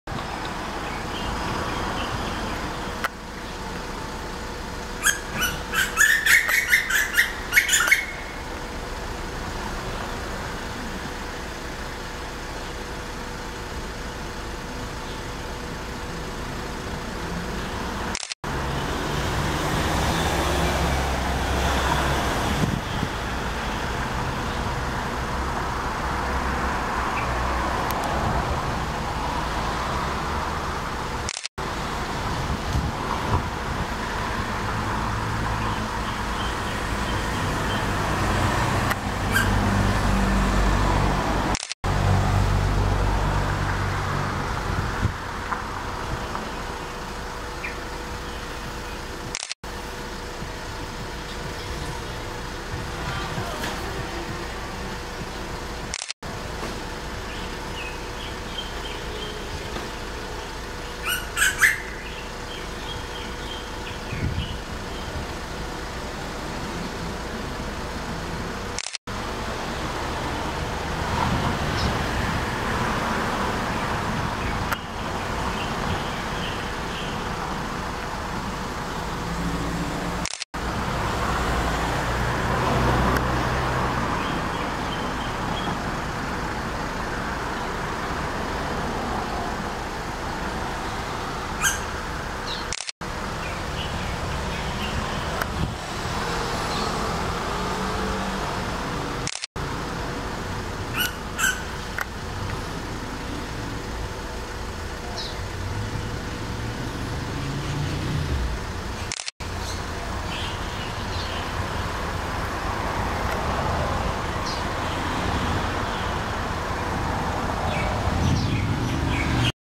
Asian koels in Singapore
Female Asian koels seen at Sembawang Hill Estate on 6 February 2021, 12.55 pm